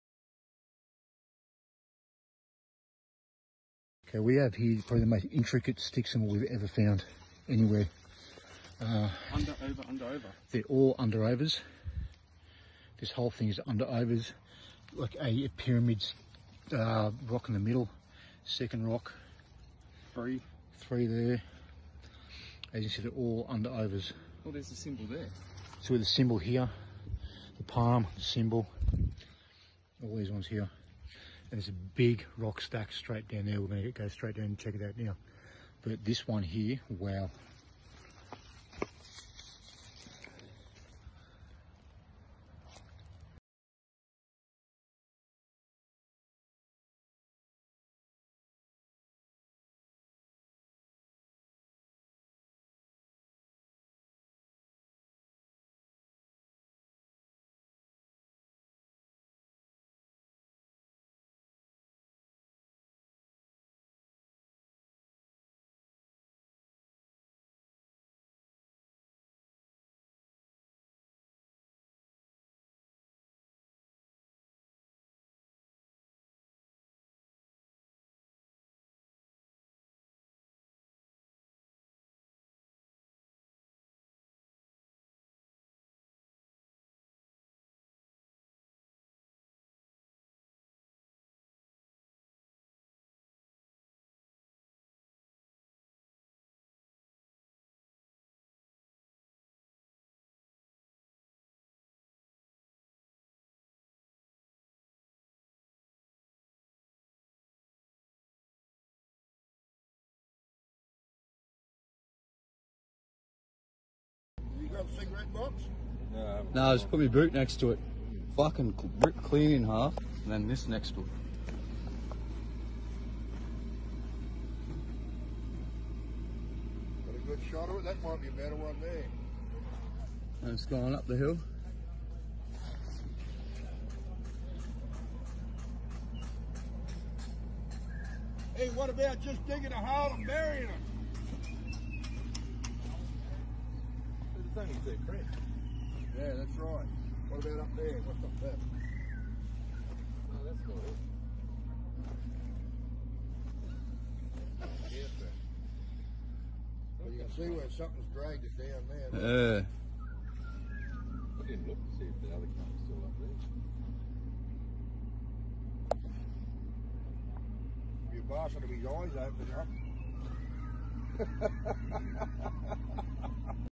S3 Ep11: PATREON BONUS CONTENT
BONUS CONTENT! See the evidence that Gary discussed with me during our interview… this is huge! (All videos and photos were used with permission from AYR)
Gary from the Australian Yowie Research group joins me today, to discuss what the organization has uncovered (with photos and videos!), personal encounters, wild reports that they have received, and Gary also shares information about an upcoming expedition that hasn't been revealed until now! This may be one of the most revealing episodes we've ever had on the show!